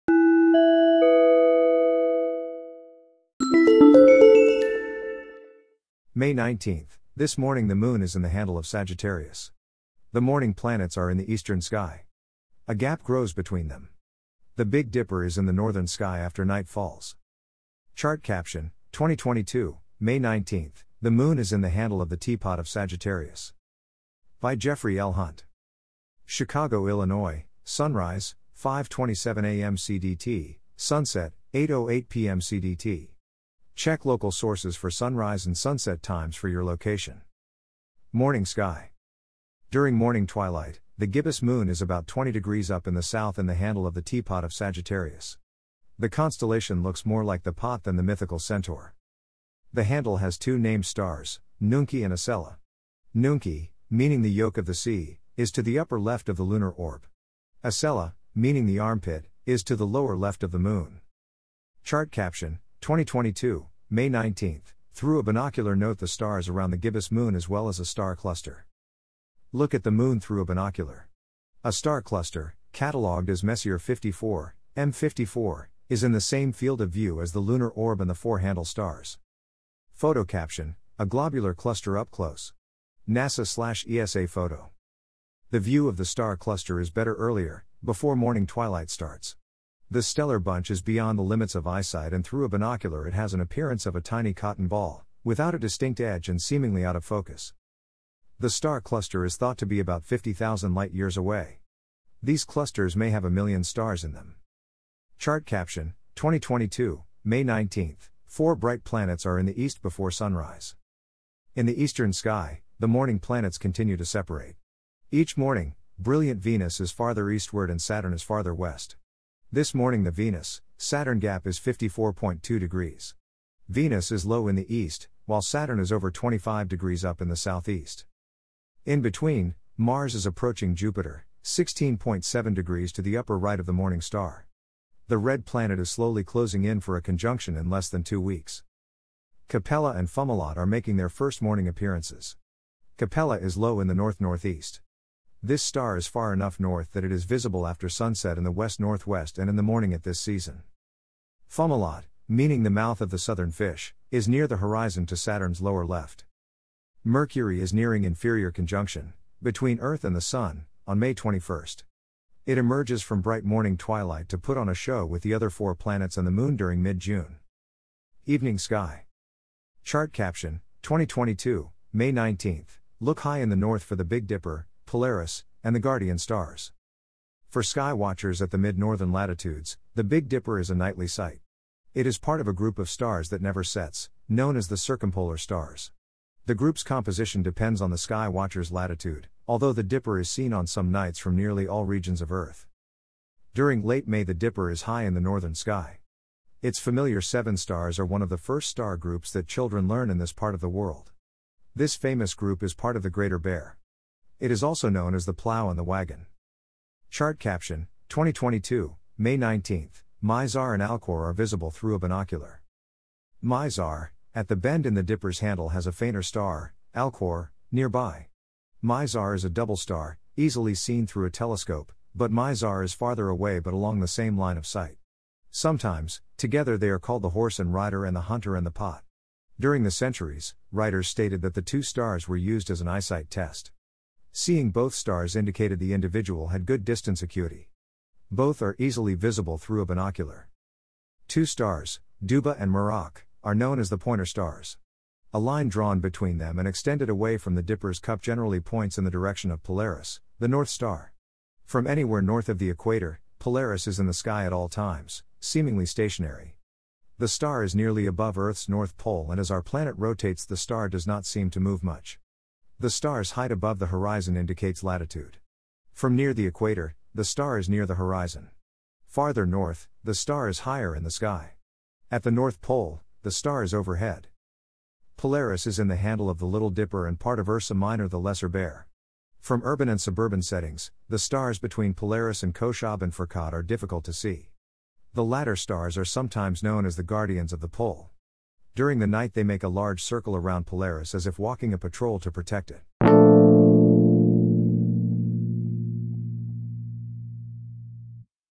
2022, May 19: Morning Planets Separate, Evening Big Dipper
May 19: This morning the moon is in the handle of Sagittarius.  The morning planets are in the eastern sky.  A gap grows between them.  The Big Dipper is in the northern sky after night falls.

This episode is also available as a blog post: 2022, May 19: Morning Planets Separate, Evening Big Dipper